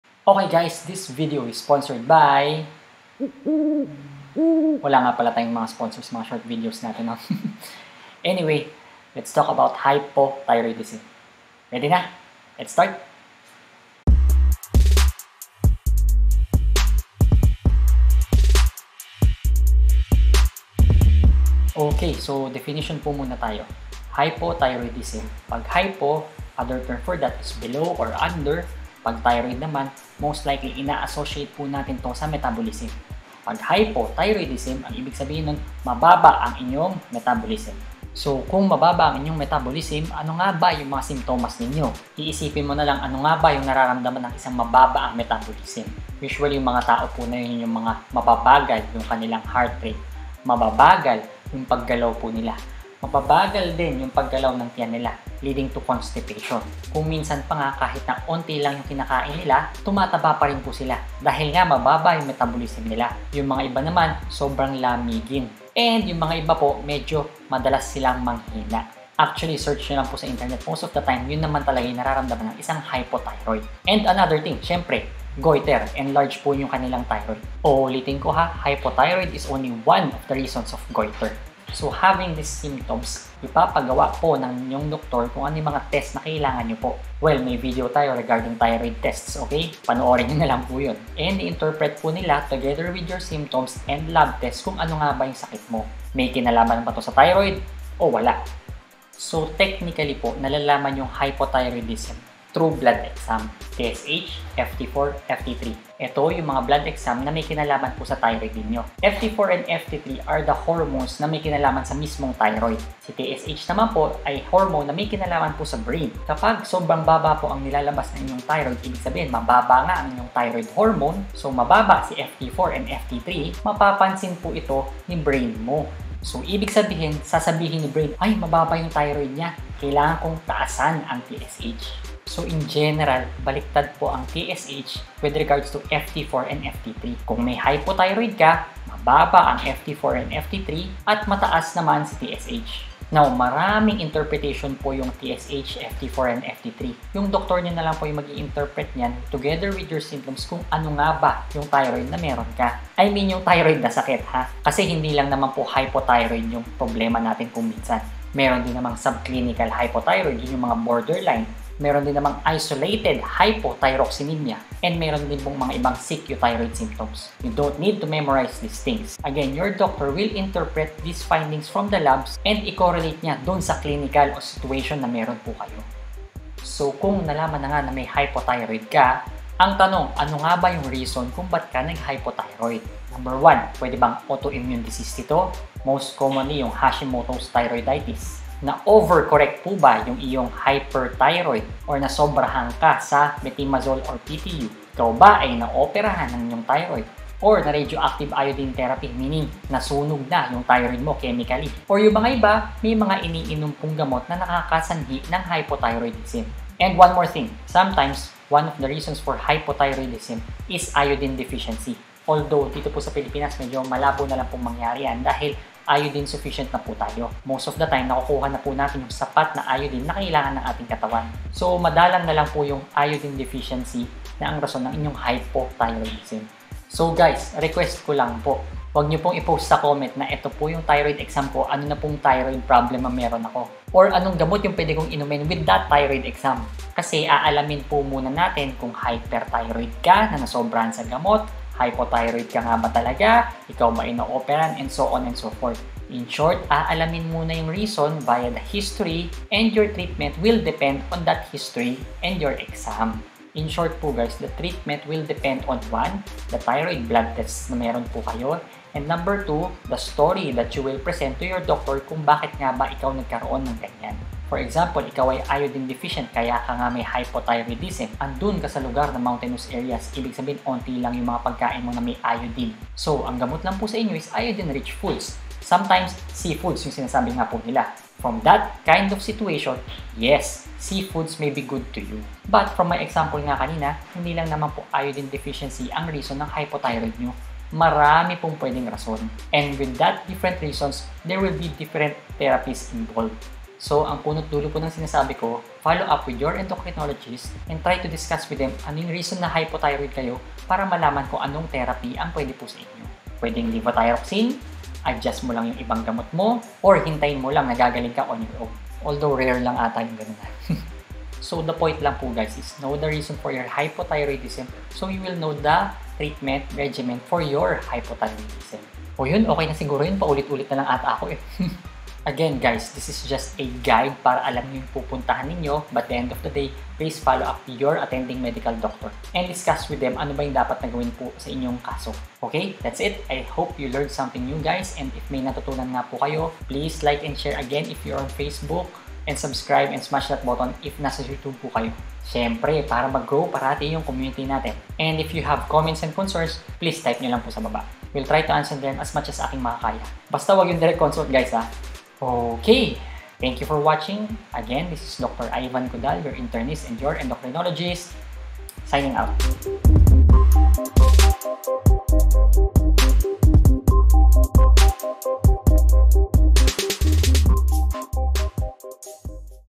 Hypothyroidism
What are the symptoms, how do we diagnose and what are possible reasons why did you have that thyroid disorder?